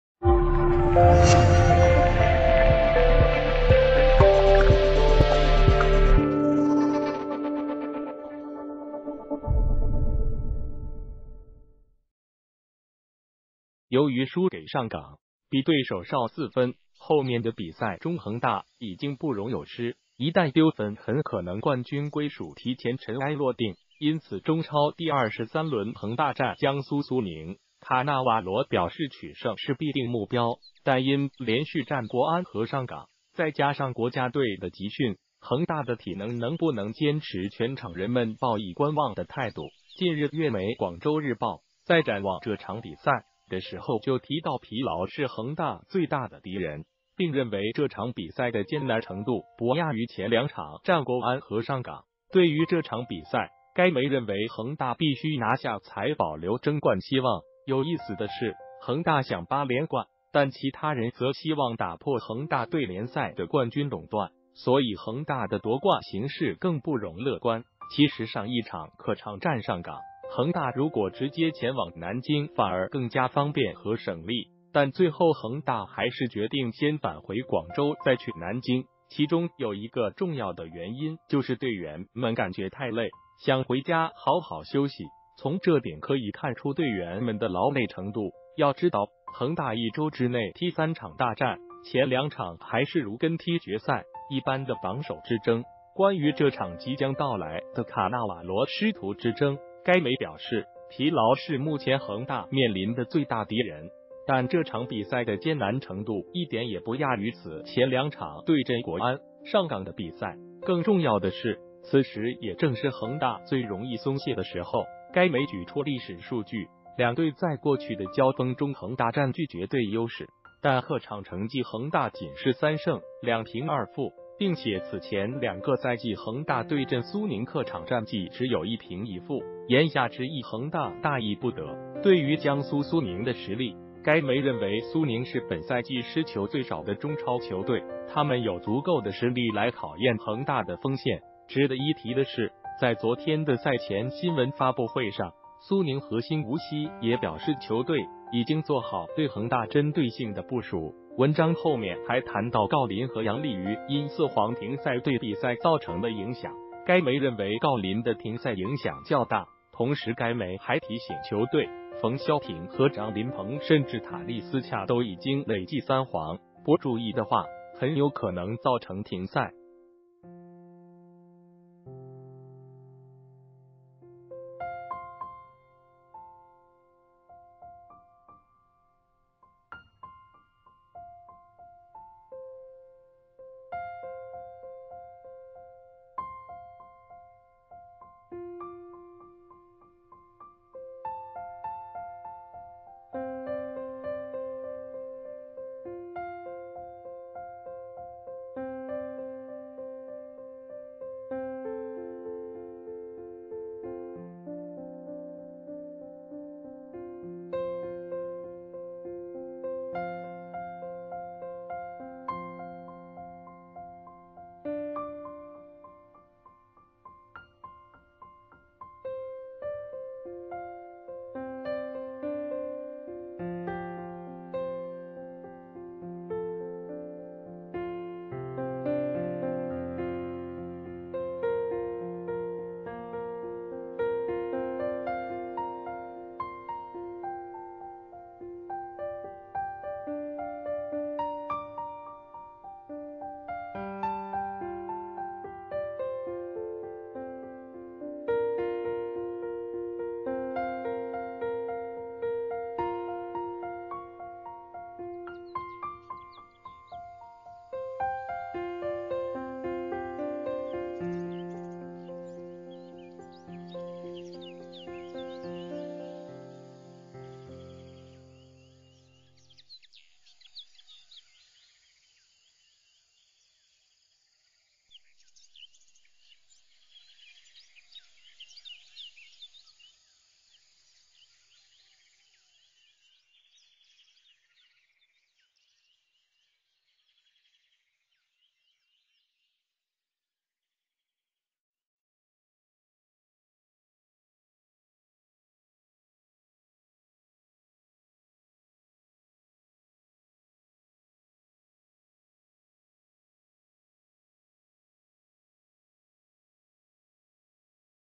恒大VS苏宁？粤媒：疲劳是最大敌人，艰难程度堪比战国安和上港！
谢谢！由于输给上港，比对手少4分，后面的比赛中恒大已经不容有失，一旦丢分很可能冠军归属提前尘埃落定。因此中超第23轮恒大战江苏苏宁，卡纳瓦罗表示取胜是必定目标。但因连续战国安和上港，再加上国家队的集训，恒大的体能能不能坚持全场人们报以观望的态度。近日粤媒《广州日报》在展望这场比赛的时候就提到疲劳是恒大最大的敌人，并认为这场比赛的艰难程度不亚于前两场战国安和上港。对于这场比赛，该媒认为恒大必须拿下才保留争冠希望。有意思的是，恒大想八连冠，但其他人则希望打破恒大对联赛的冠军垄断，所以恒大的夺冠形势更不容乐观。其实上一场客场战上港，恒大如果直接前往南京反而更加方便和省力，但最后恒大还是决定先返回广州再去南京。其中有一个重要的原因就是队员们感觉太累，想回家好好休息。从这点可以看出队员们的劳累程度。要知道，恒大一周之内踢三场大战，前两场还是如跟踢决赛一般的榜首之争。关于这场即将到来的卡纳瓦罗师徒之争，该媒表示：“疲劳是目前恒大面临的最大敌人，但这场比赛的艰难程度一点也不亚于此前两场对阵国安、上港的比赛。更重要的是，此时也正是恒大最容易松懈的时候。”该媒举出历史数据：“两队在过去的交锋中恒大占据绝对优势，但客场成绩恒大仅是3胜2平2负，并且此前两个赛季恒大对阵苏宁客场战绩只有1平1负。”言下之意恒大大意不得。对于江苏苏宁的实力，该媒认为苏宁是本赛季失球最少的中超球队，“他们有足够的实力来考验恒大的锋线。”值得一提的是，在昨天的赛前新闻发布会上，苏宁核心吴曦也表示球队已经做好对恒大针对性的部署。文章后面还谈到郜林和杨立瑜因4黄停赛对比赛造成的影响，该媒认为郜林的停赛影响较大。同时该媒还提醒球队，冯潇霆和张琳芃甚至塔利斯卡都已经累计3黄，不注意的话很有可能造成停赛。